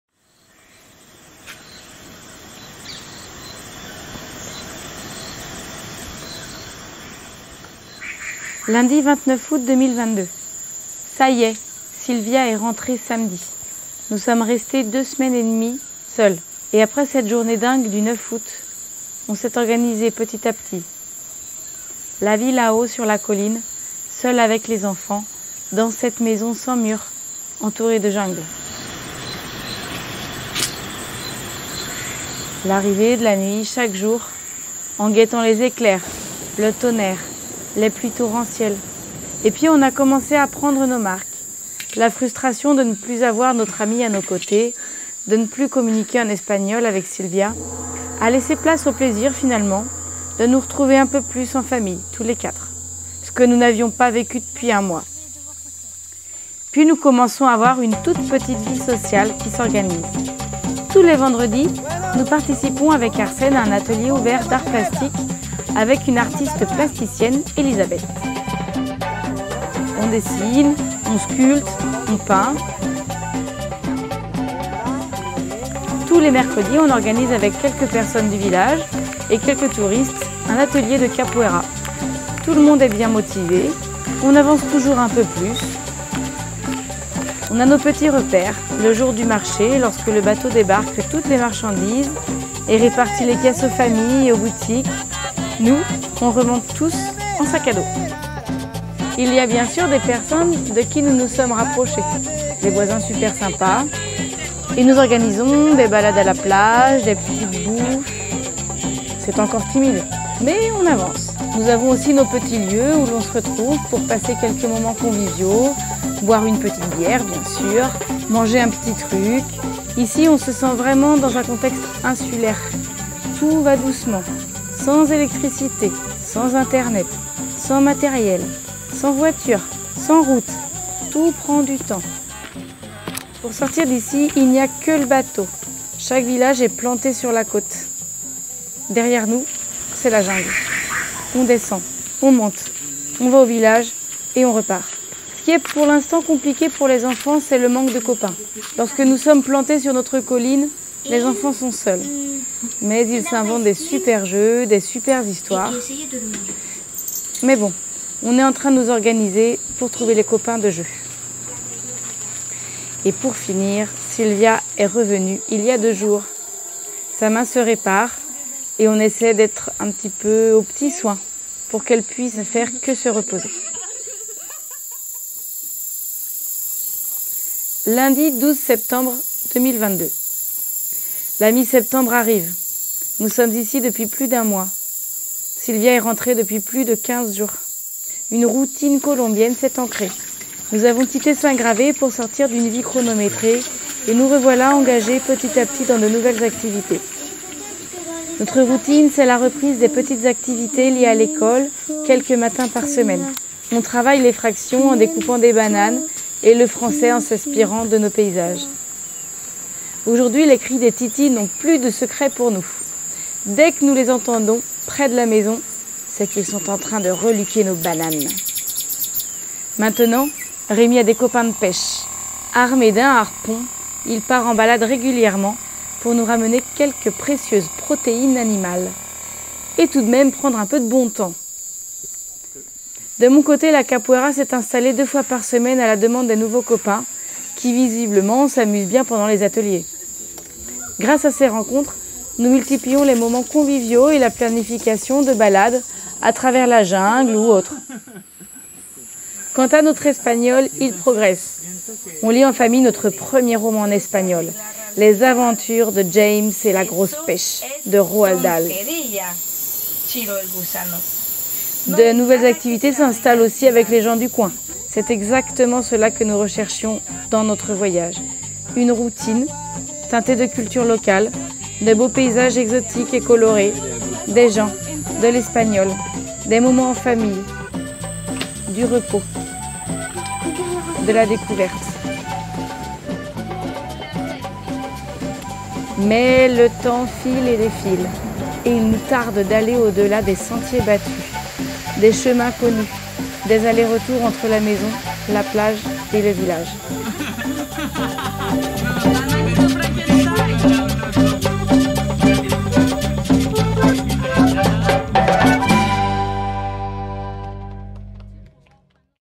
Mon Rêve sous les Tropiques #7
Quoi de mieux qu'un carnet pour accompagner une expérience comme la notre ? C'est par ce biais que l'on vous présente, dans cette septième chronique de notre voyage, notre vie à San Francisco.